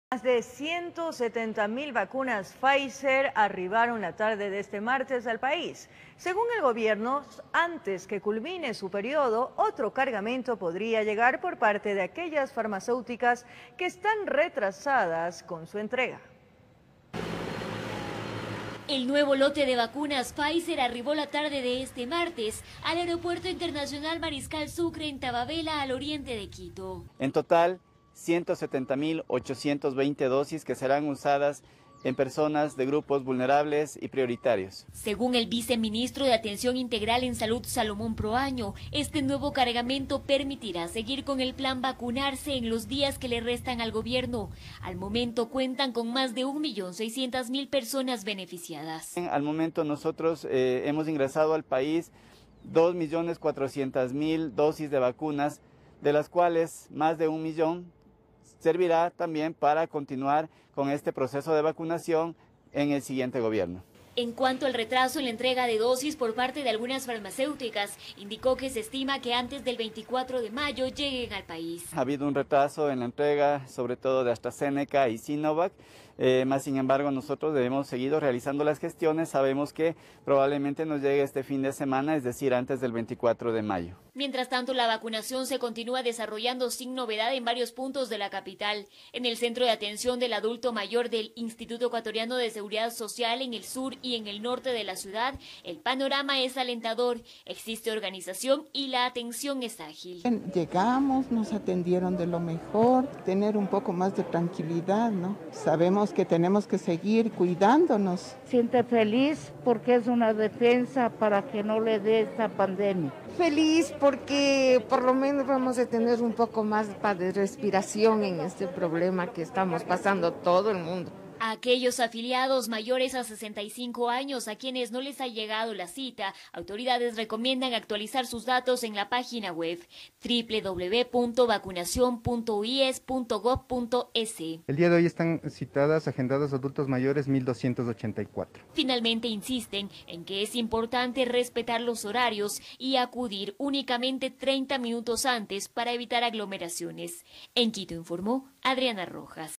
Nuevo cargamento de vacunas Pfizer llegó al país
Más de 170.000 vacunas Pfizer arribaron la tarde de este martes al país.